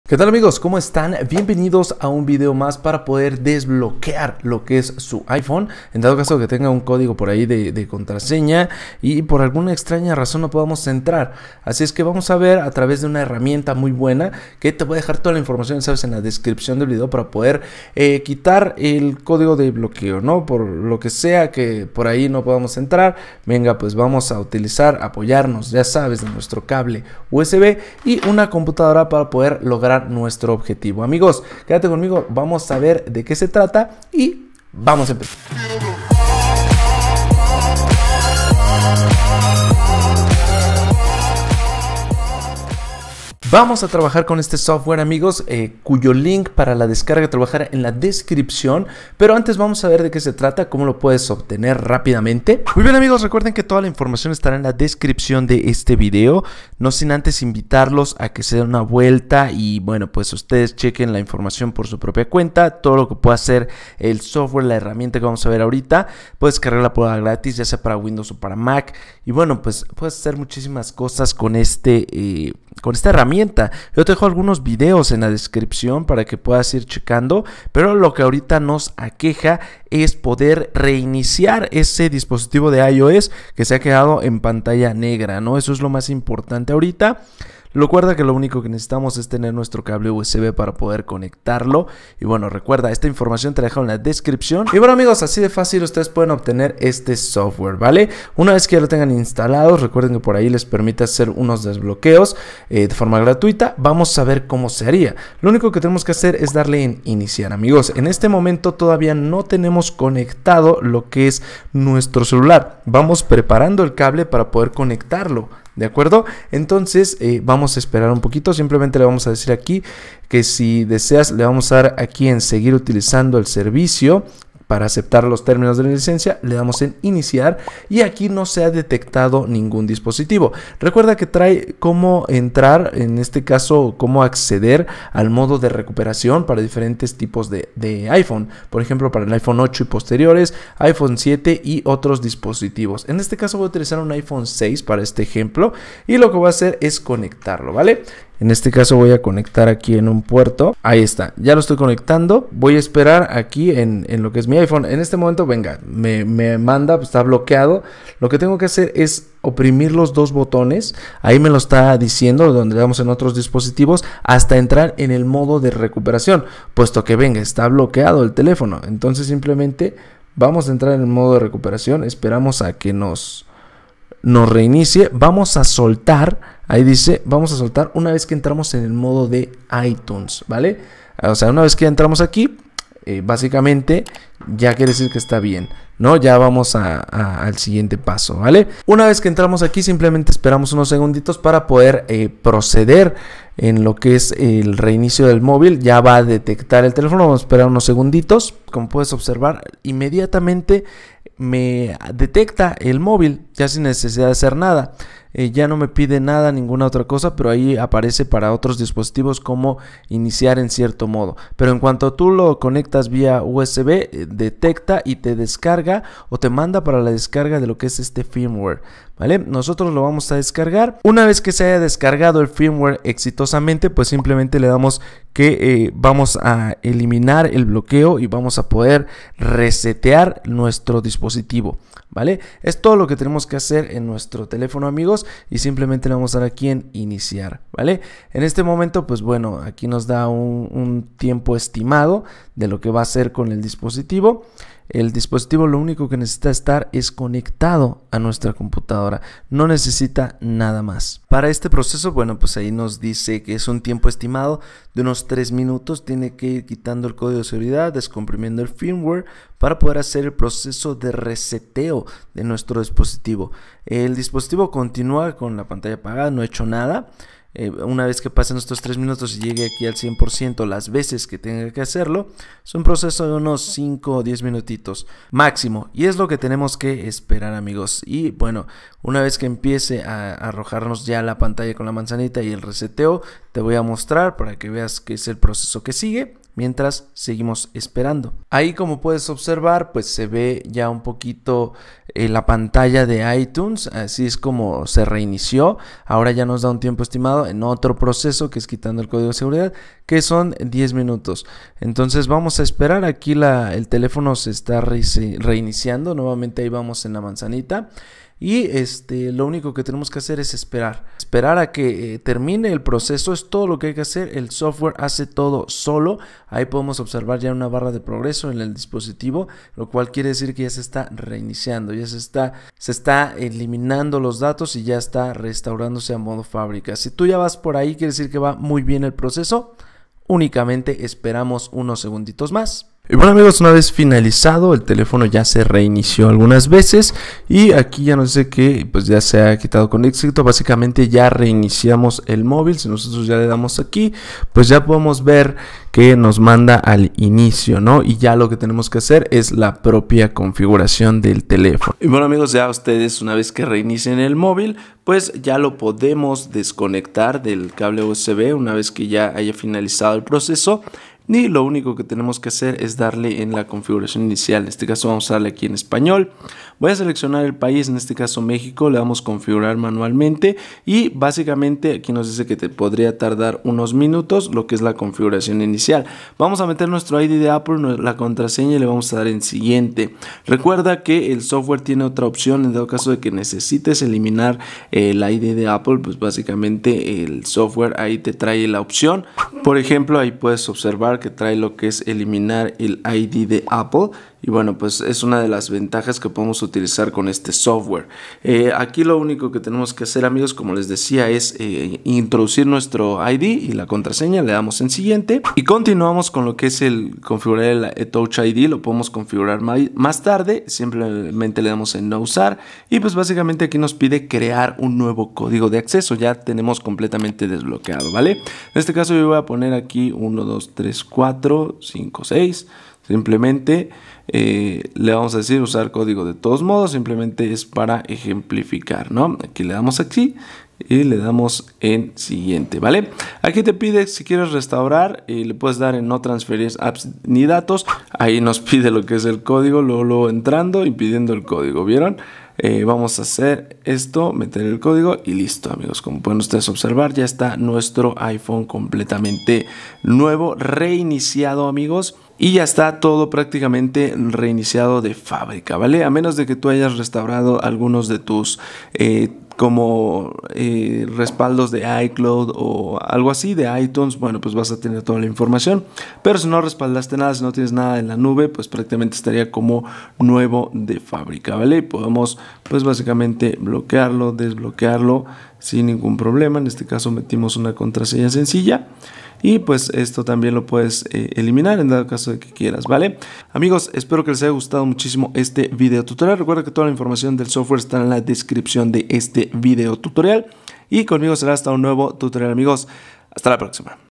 COMO DESBLOQUEAR UN IPHONE 13/12/11/x/8/7/6 Por si olvidas tu contraseña 2022 | Tutorial en Windows
En este video tutorial te mostrare como quitar el bloqueo de pantalla en iPhone, en cualquier versión de forma fácil y rápida.
Un click para eliminar el código de desbloqueo del iPhone y iPad. Eliminar el Apple ID sin necesidad de conocer la contraseña de tu dispositivo.

Este video solo muestra como desbloquear tu iphone de tu propiedad si se te ha olvidado la contraseña.